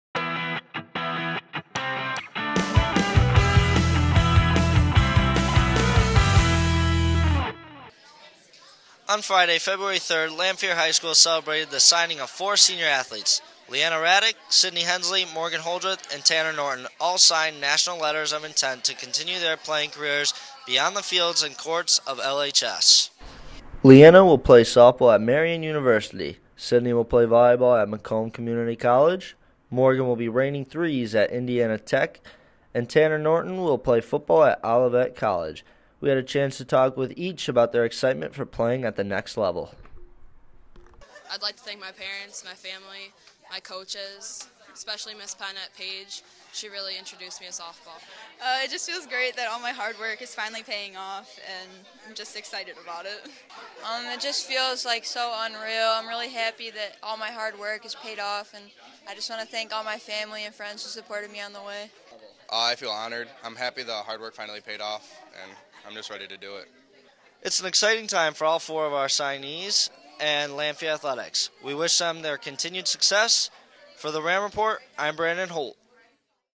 Signing day
Last week four Lamphere students signed on to college teams.